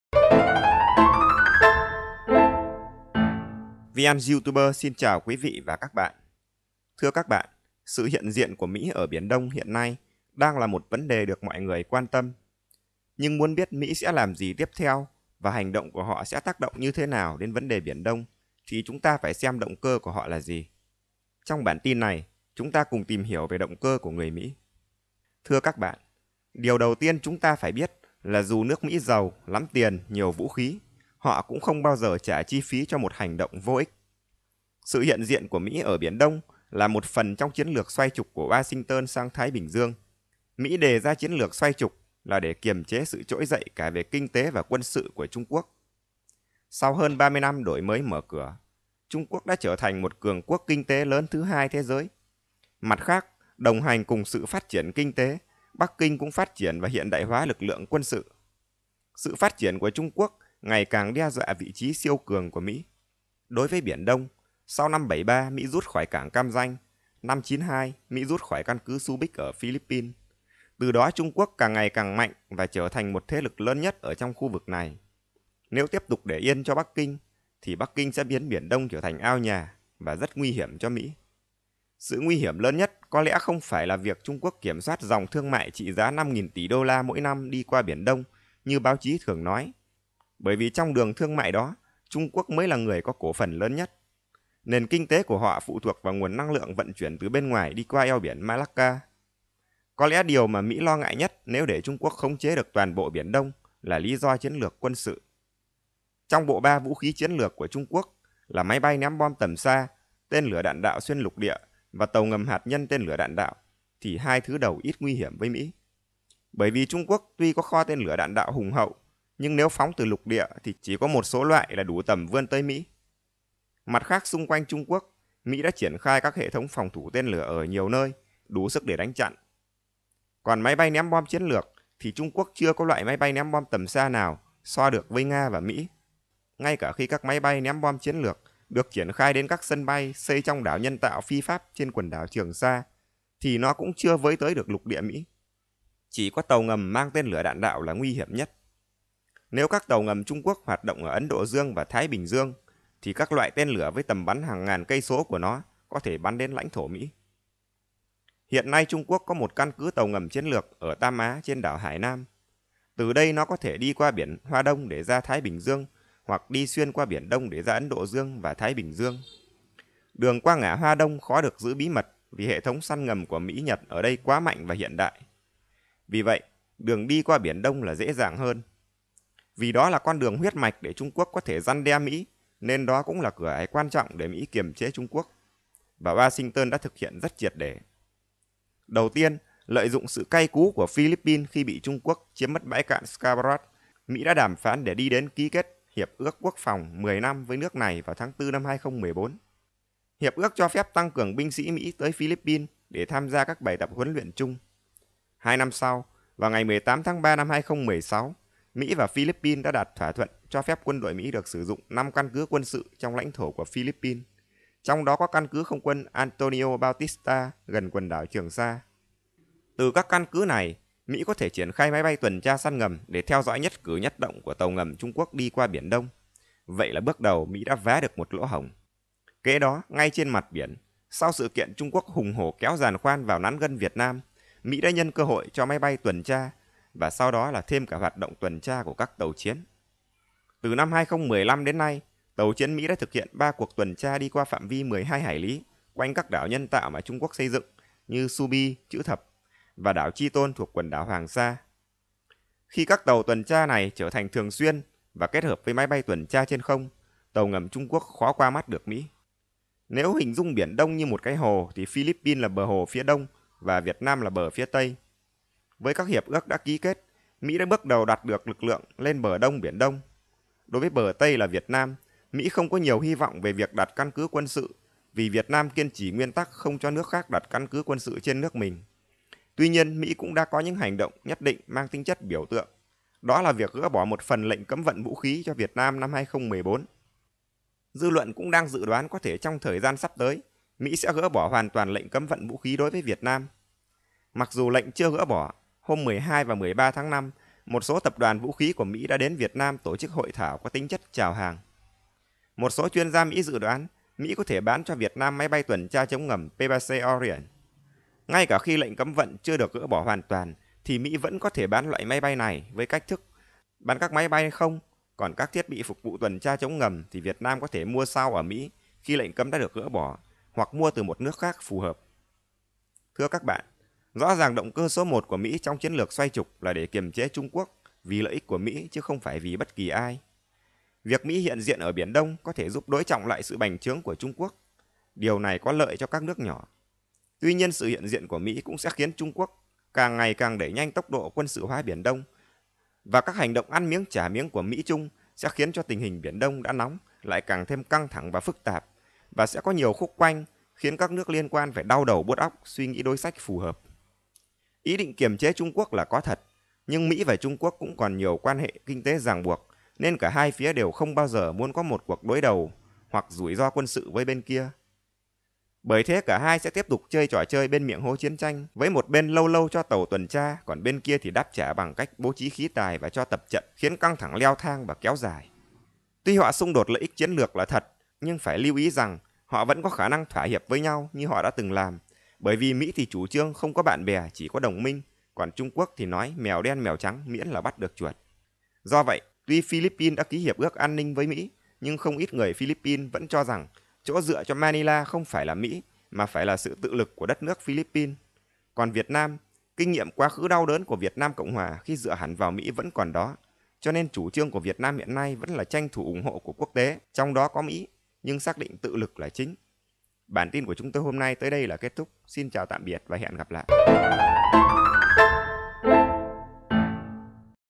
Mỹ có mục đích gì thật sự ở Biển Đông? (YTB-32)
Không phải là lợi ích trong dòng thương mại 5000 tỷ đô la chạy qua Biển Đông. Mục đích thật sự của Mỹ trong việc triển khai hiện diện quân sự ở Biển Đông là để chặn cửa ngõ ra Thái Bình Dương của các tàu ngầm Trung Quốc.